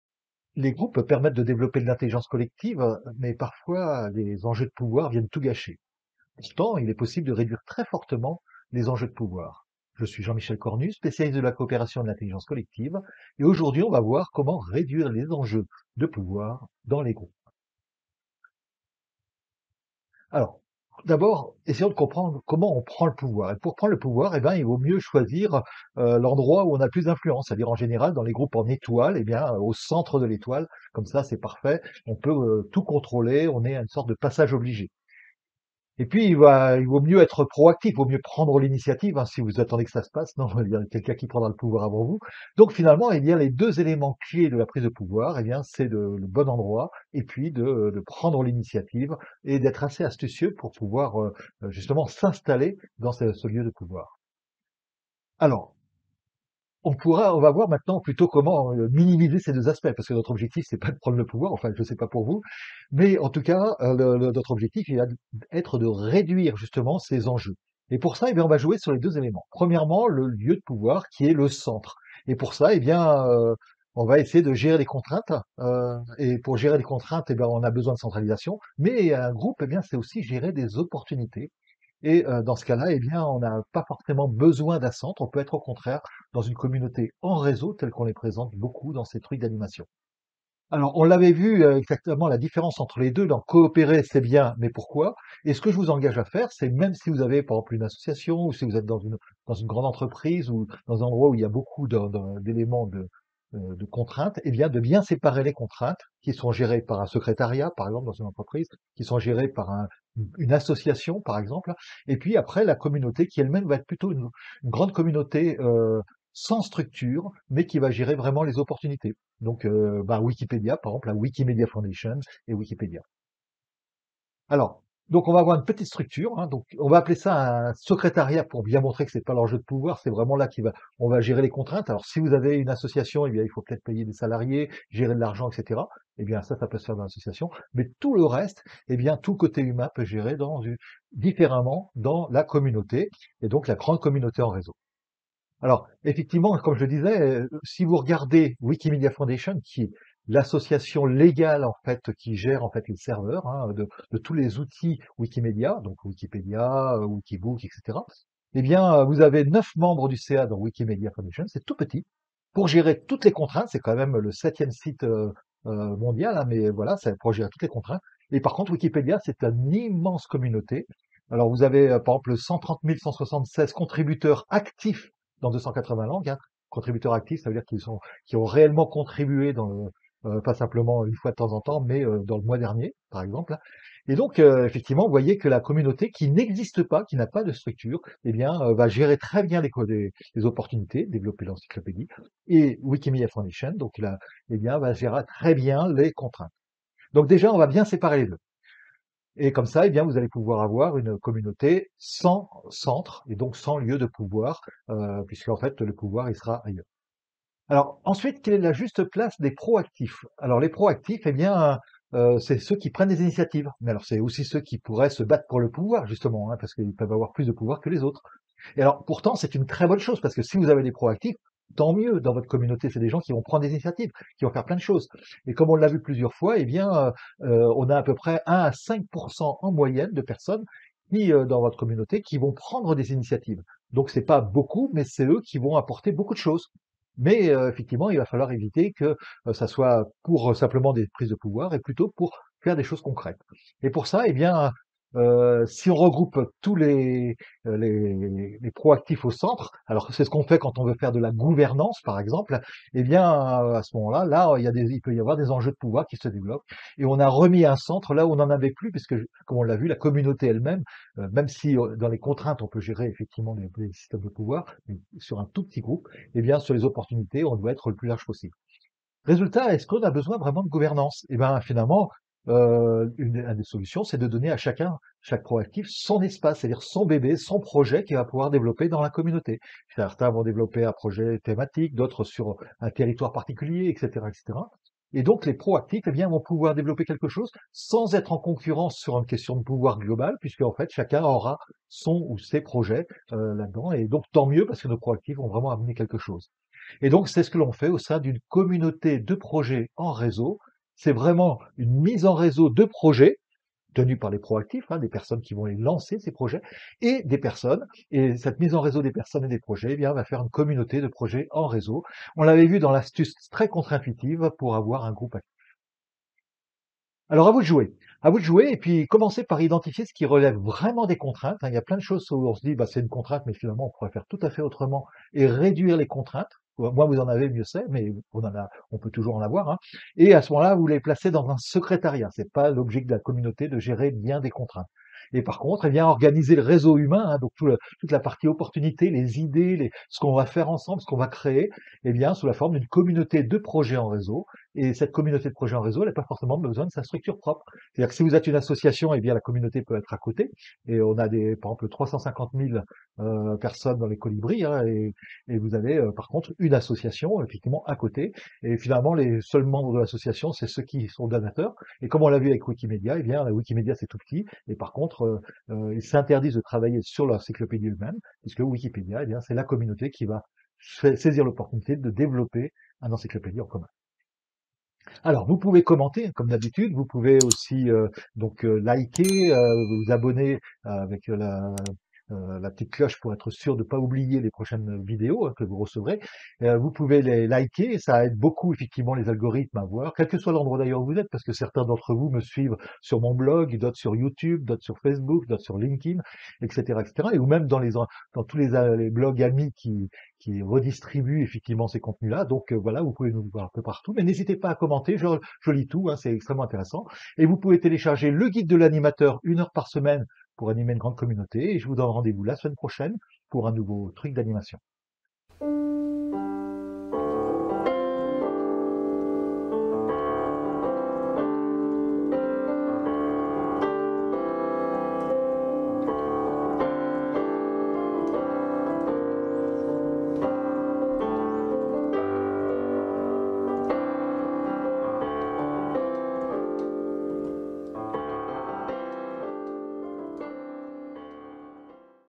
Réduire les enjeux de pouvoir dans les groupes
Les groupes permettent de développer de l'intelligence collective sur le long terme, mais parfois les enjeux de pouvoir viennent tout gâcher... Pourtant il est possible de s'organiser pour réduire très fortement  les luttes de pouvoir !